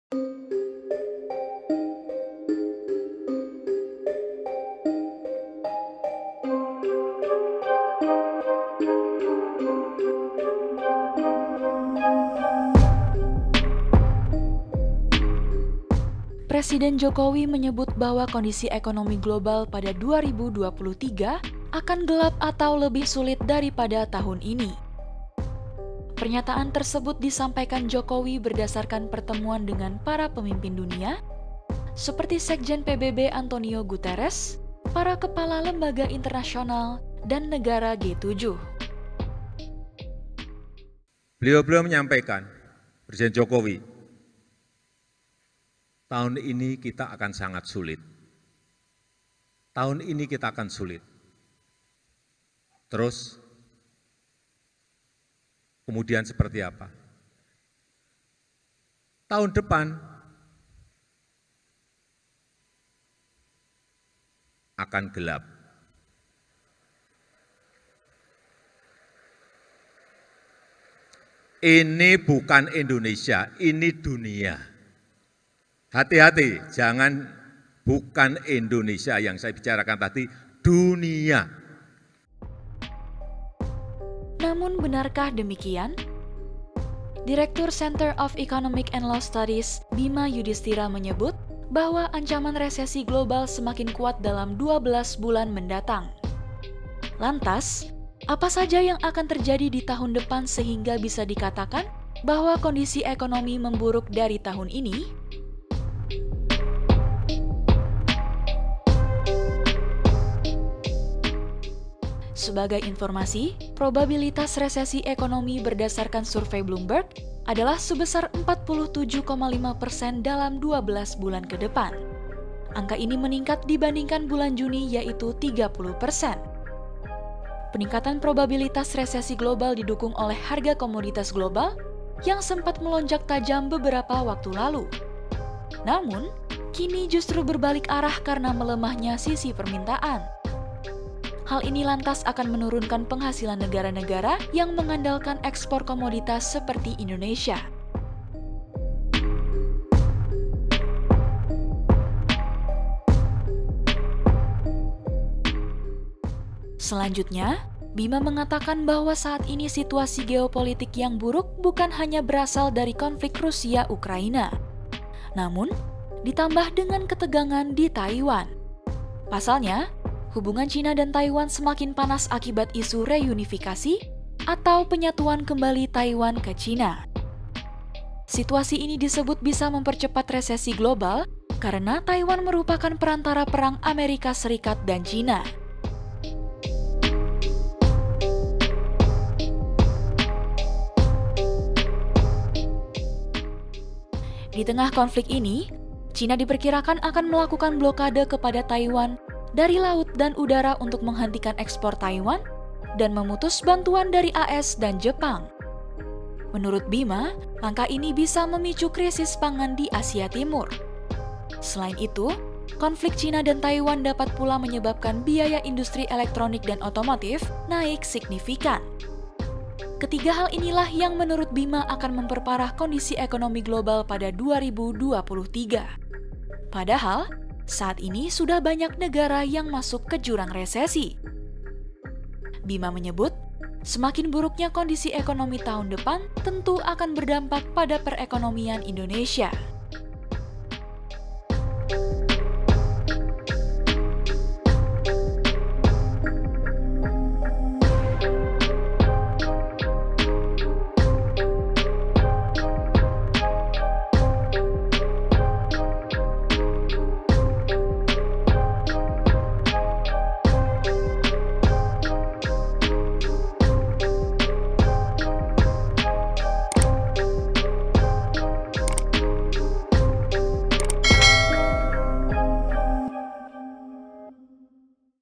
Jokowi Sebut Ekonomi Global Tahun Depan Akan Gelap, Benarkah?
Presiden Jokowi menyebutkan bahwa kondisi ekonomi global pada 2023 akan gelap atau lebih sulit daripada tahun ini.

Pernyataan tersebut disampaikan Jokowi berdasarkan pertemuan dengan para pemimpin dunia, seperti Sekjen PBB Antonio Guterres, para kepala lembaga internasional, dan negara G7.
Namun, benarkah demikian?

Direktur Center of Economic and Law Studies (CELIOS) Bhima Yudhistira menyebut bahwa ancaman resesi global semakin kuat dalam 12 bulan mendatang.

Lantas, apa saja yang akan terjadi di tahun depan sehingga bisa dikatakan bahwa kondisi ekonomi memburuk dari tahun ini?

Video Jurnalis: Arie Julianto
Penulis: Isna Rifki Sri Rahayu
Penulis Naskah: Alexandra Birgitta Anandaputri
Narator: Alexandra Birgitta Anandaputri
Video Editor: Alexandra Birgitta Anandaputri
Produser: Khairun Alfi Syahri MJ